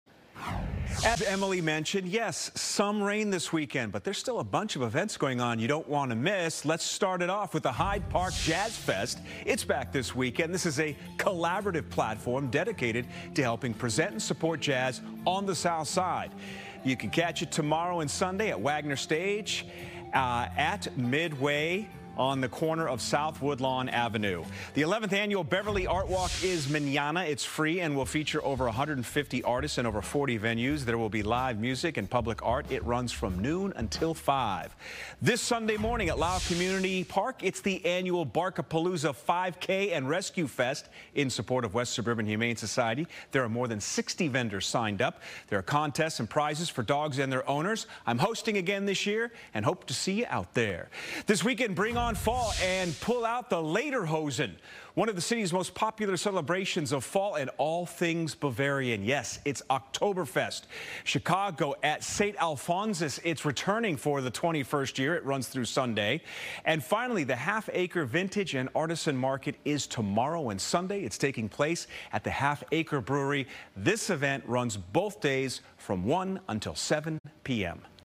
Weekend in Chicago: Hyde Park Jazz Festival, Barkapalooza
We may have some rain this weekend in the Chicago area, but there's still plenty of events going on that you won't want to miss.